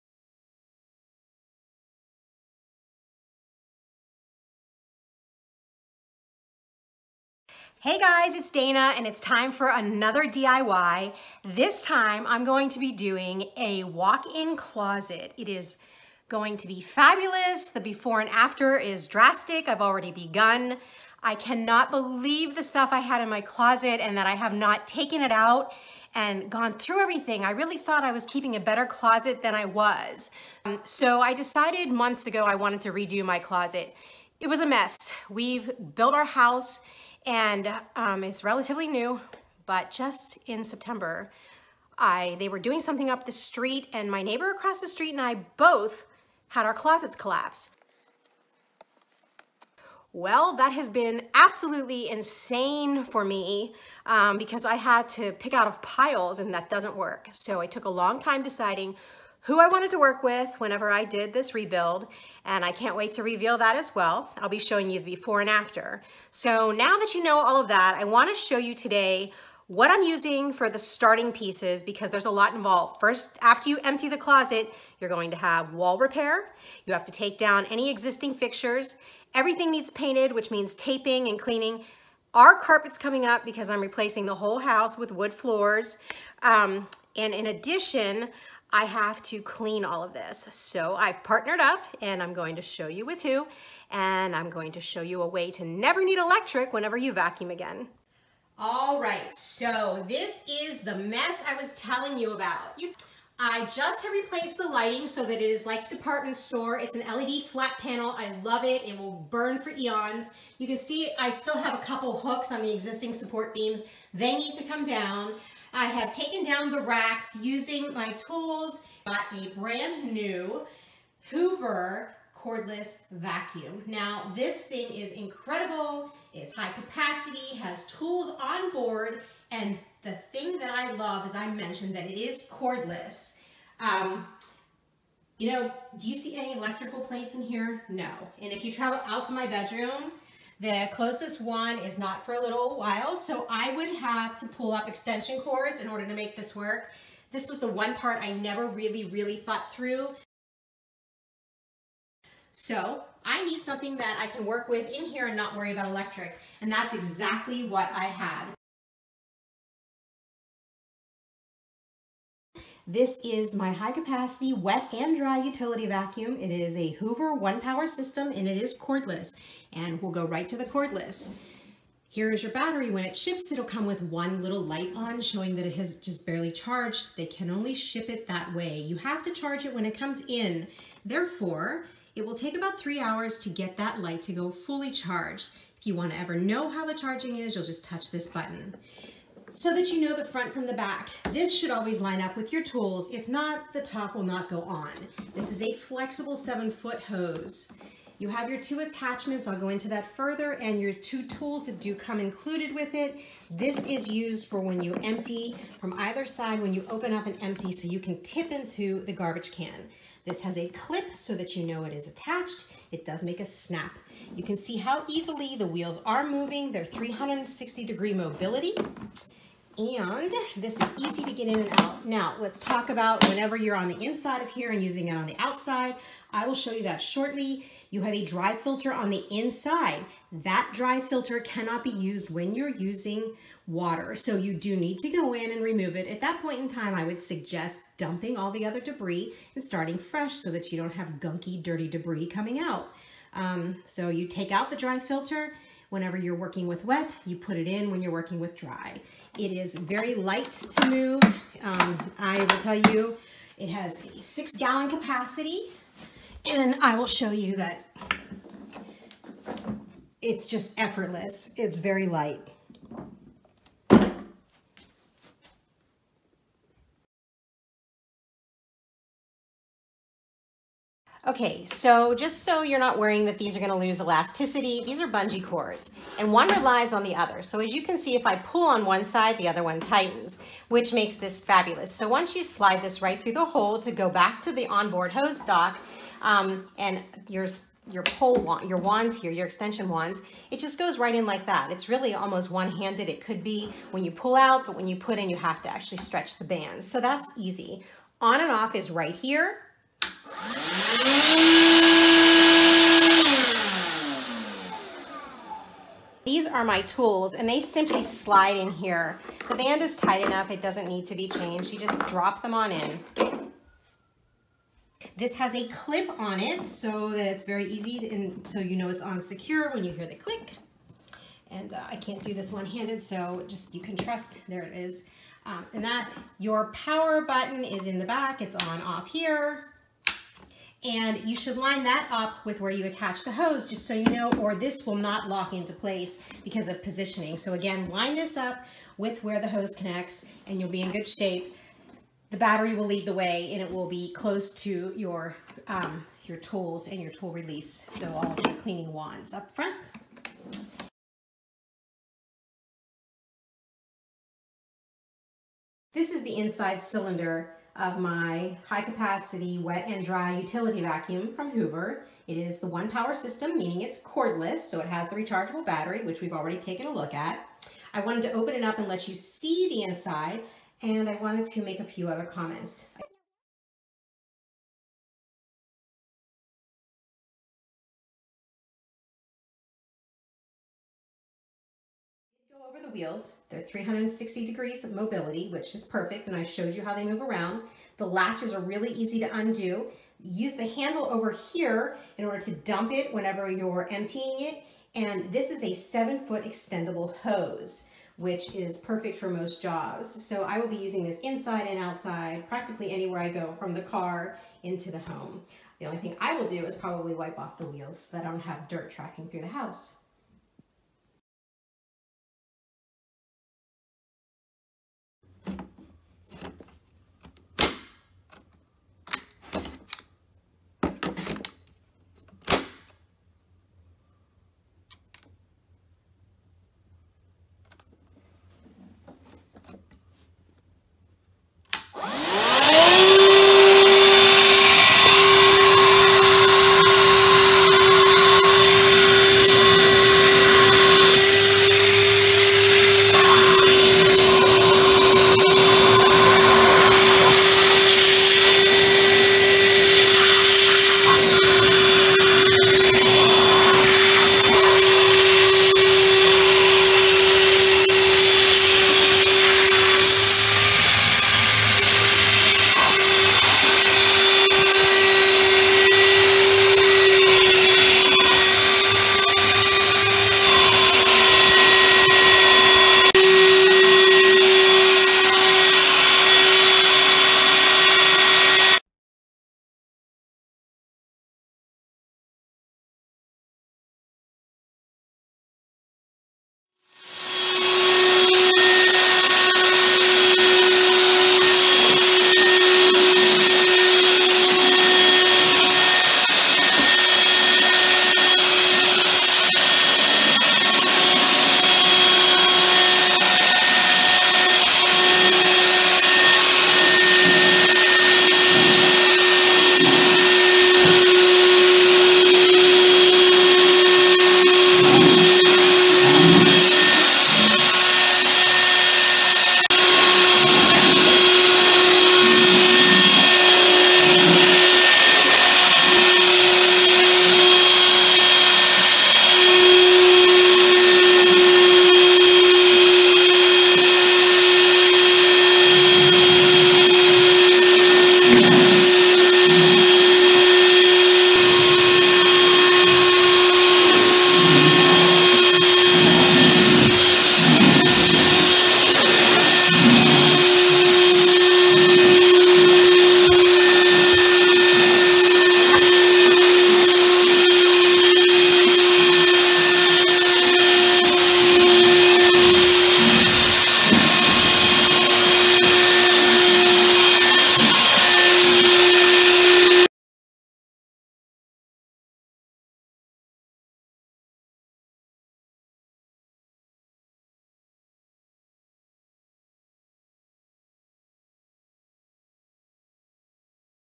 Cordless Utility Vacuum Means No Extension Cords!
(product provided) I am starting to renovate my walk-in closet. As I do, I am using the HOOVER® ONEPWR™ Cordless High-Capacity Utility Vacuum which requires only a battery pack! This means, no more extension cords when I work! No tripping, and no worrying if the location has NO ELECTRIC. My vacuum is charged and ready to go!